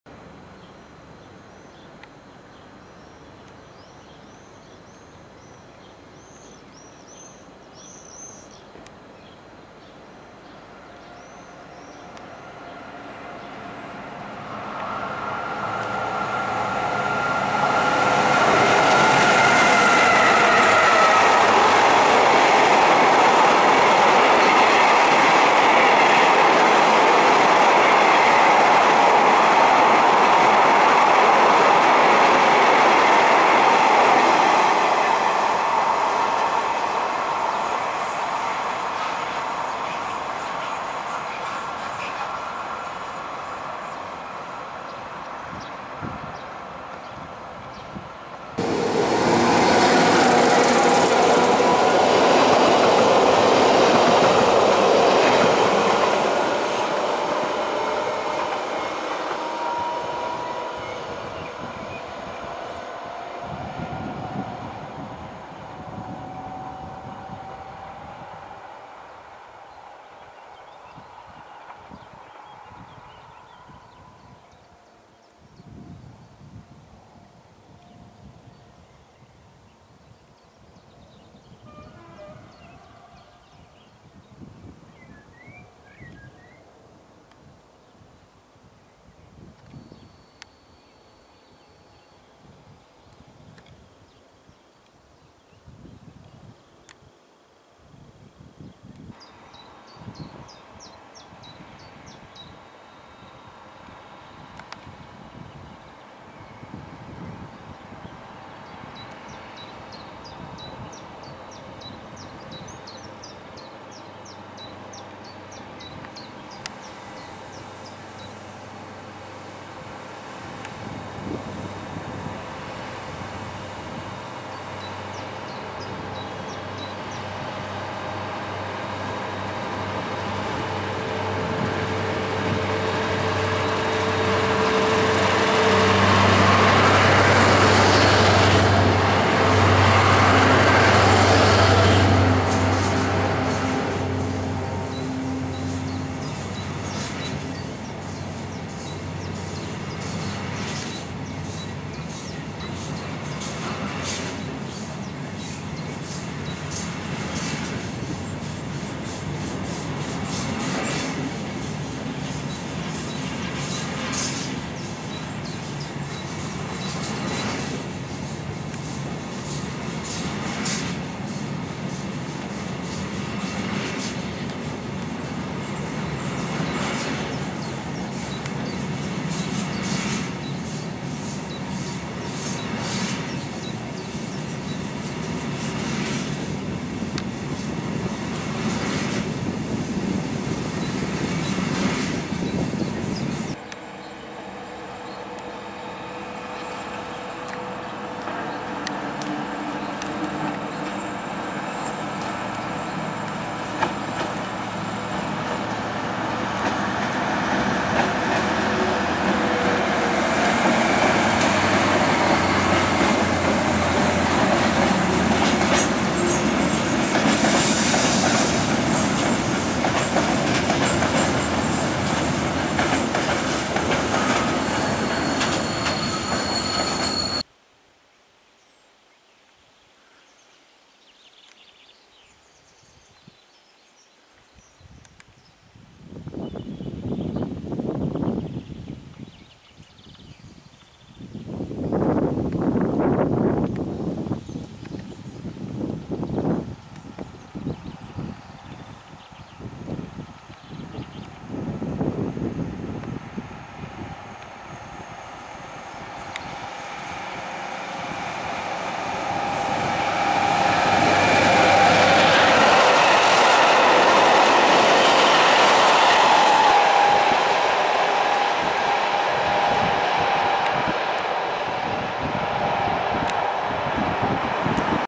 Trains Fret Sncf entre Rennes et Saint-Brieuc
Trains Frets Sncf, céréales et Citernes.
UM BB 60000 et US BB 75000 entre Rennnes et St Brieuc.